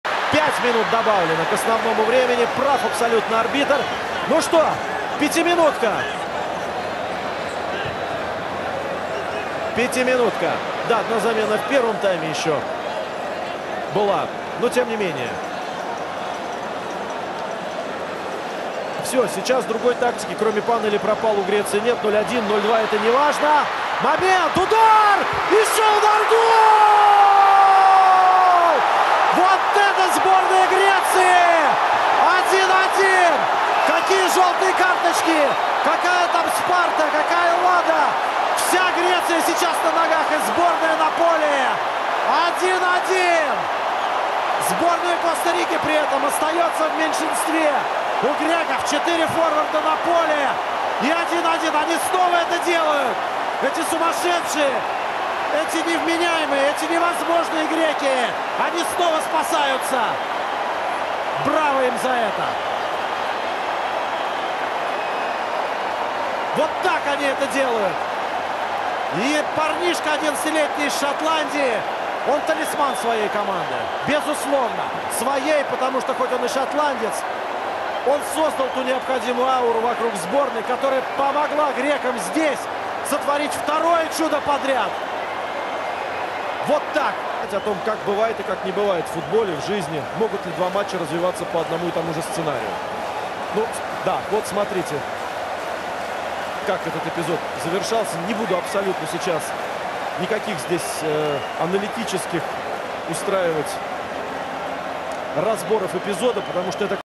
World Cup 2014 Costa Rica Greece 1 1 Sokratis Papastathopoulos
Коста Рика-Греция
Чемпионат мира по футболу 2014
1/8 финала

Costa Rica-Grecia
Copa Mundial de la FIFA 2014
1/8 de final

Κόστα Ρίκα-Ελλάδα
Παγκόσμιο Κύπελλο FIFA 2014
1/8 τελικοί